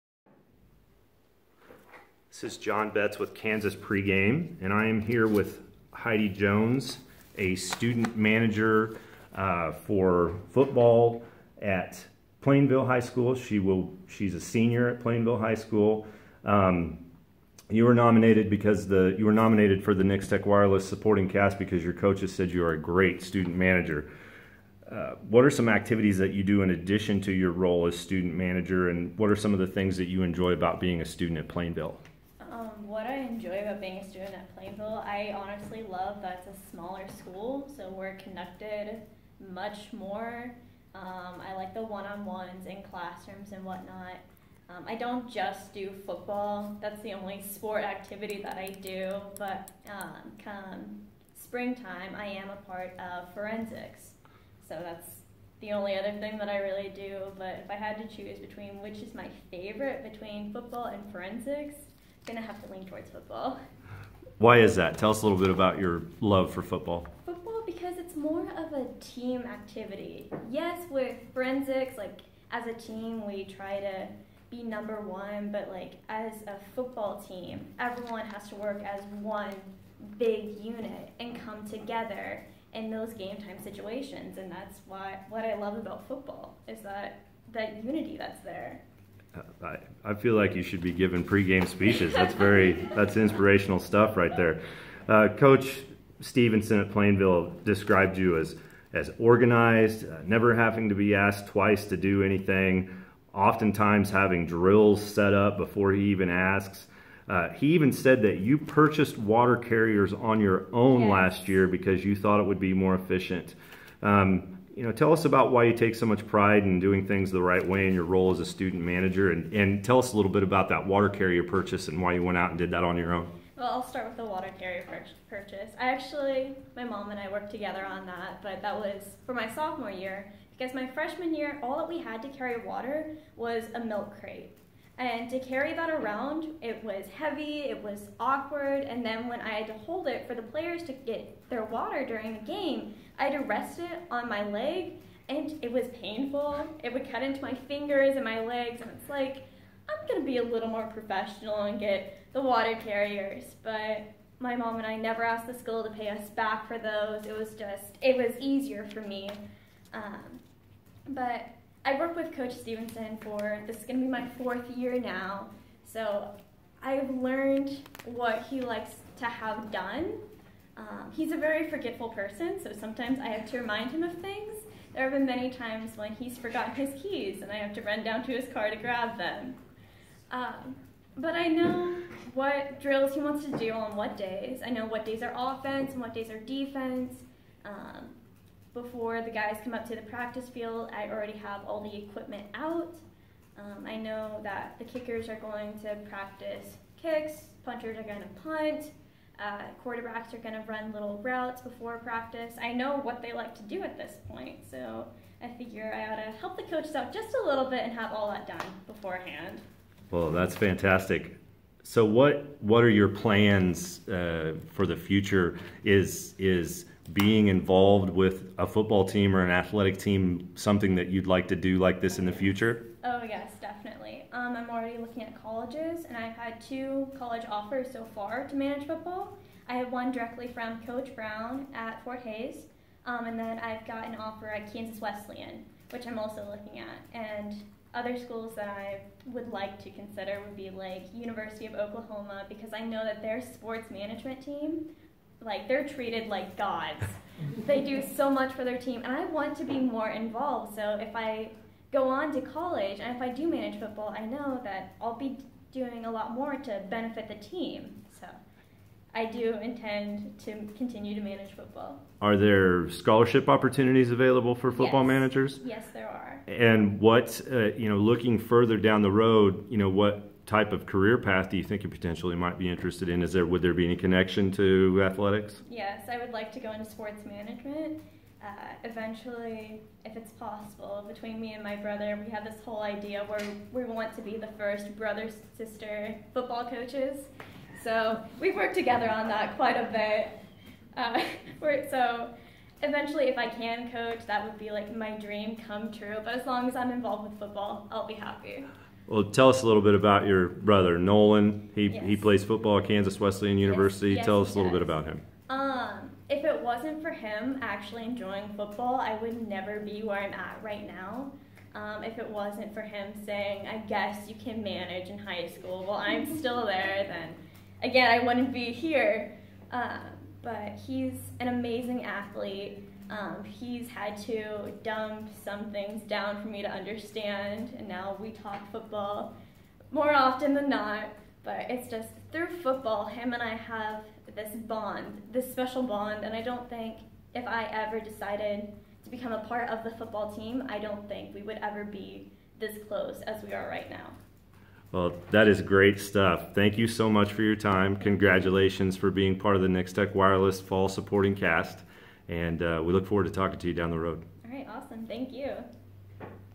Nex-Tech Wireless Supporting Cast: Heidi Jones, Plainville
Plainville senior Heidi Jones is more than just a student-manager for the football team, she is a key contributor to the success of the team.